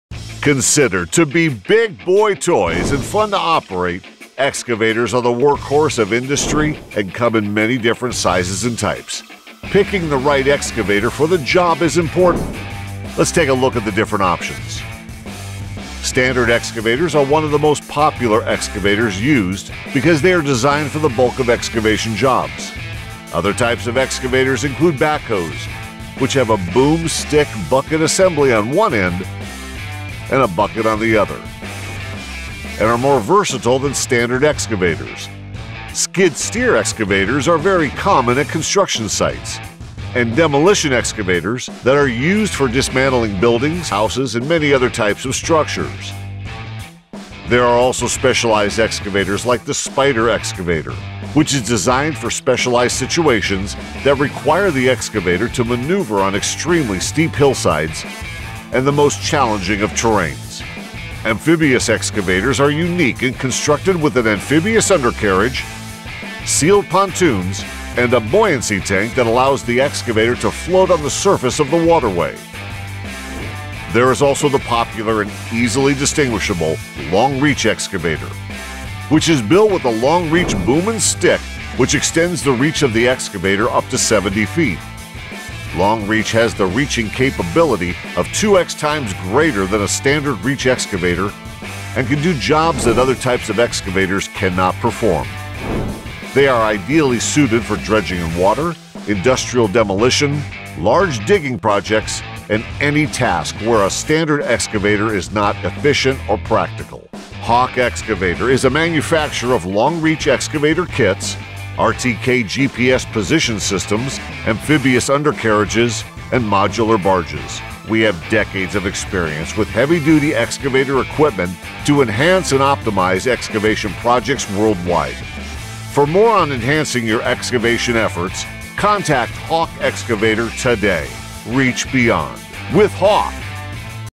Excavator 101 - Learn About the Types of Excavators and Their Uses
Excavators are heavy construction equipment that are used for many tasks and functions including digging and material handling, mining, river dredging, construction, forestry, demolition, and any other jobs that require the digging/dredging and moving of soil and other types of materials.